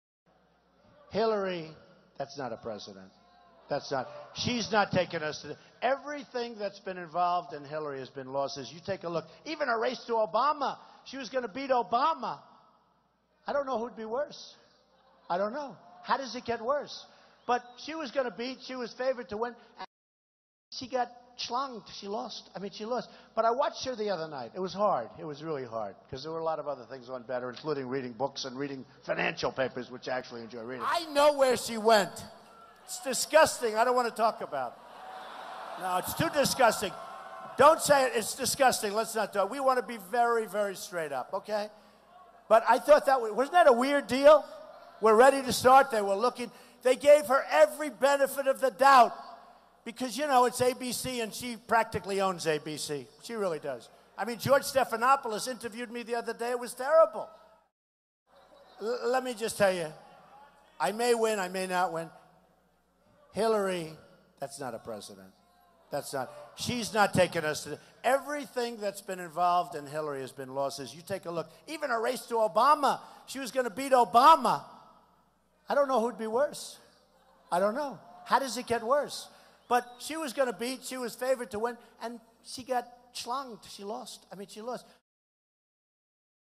WATCH: Donald Trump Launches Vulgar Attack Against Hillary Clinton
GRAND RAPIDS, Michigan — Donald Trump launched a vulgar attack on Hillary Clinton late Monday, including a sexually derogatory comment about her being "schlonged" by President Barack Obama in 2008.

"Even her race to Obama, she was gonna beat Obama," the GOP frontrunner told a rally in Grand Rapids, Michigan. "I don't know who would be worse, I don't know. How does it get worse? But she was gonna beat — she was favored to win — and she got schlonged. She lost."

Trump also made crude references to Clinton's bathroom break during Saturday's Democratic debate, describing it as "disgusting."